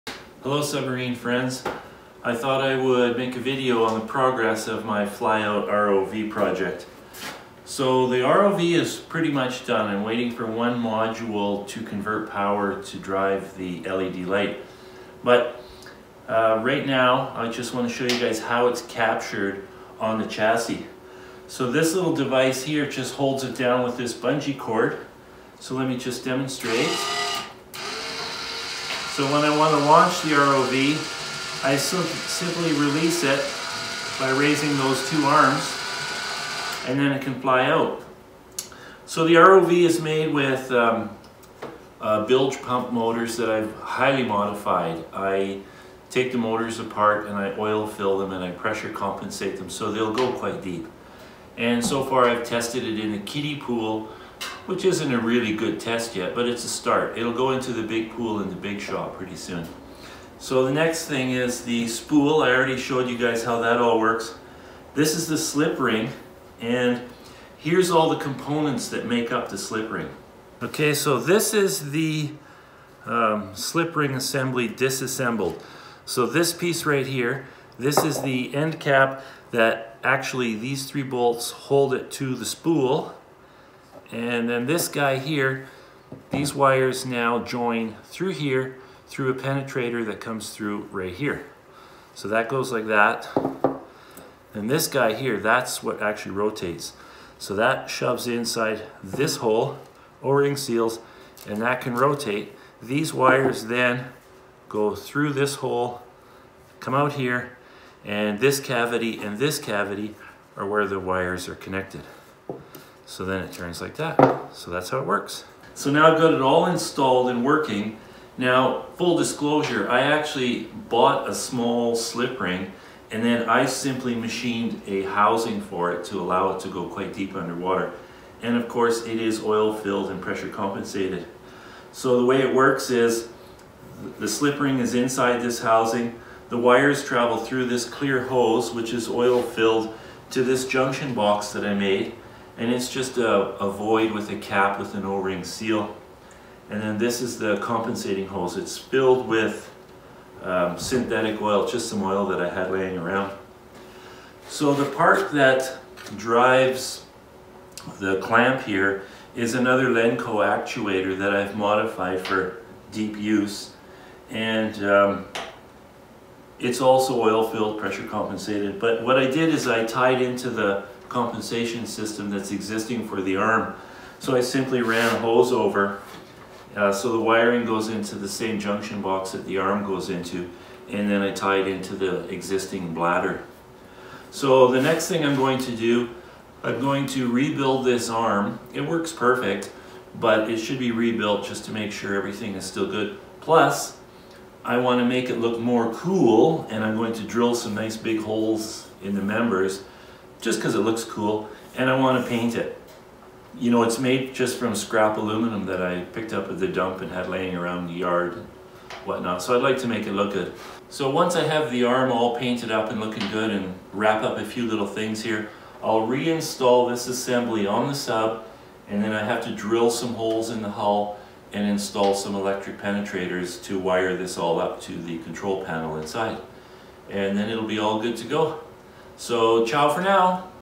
Homemade ROV Demonstration
In this video I am giving a demonstration of the new homemade ROV that rides on my homemade submarine.

If you enjoyed this video check out some of my others!